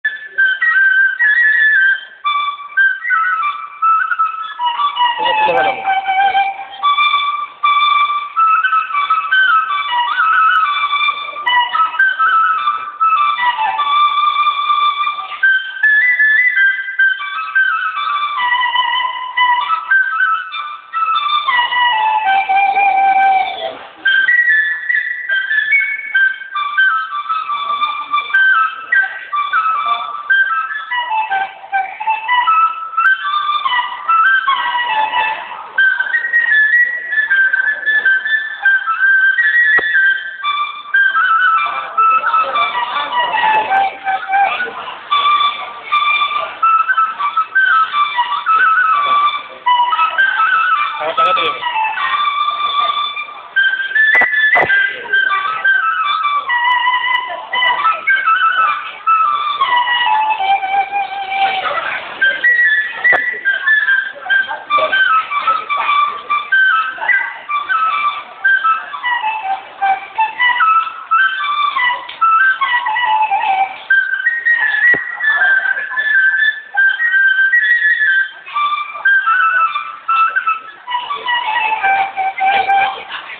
Bamboo Flute Playing By a street beggar in Shenzhen
A Begger in Shenzhen playing bamboo flute inside a underpass in Dongman of Shenzhen City/ China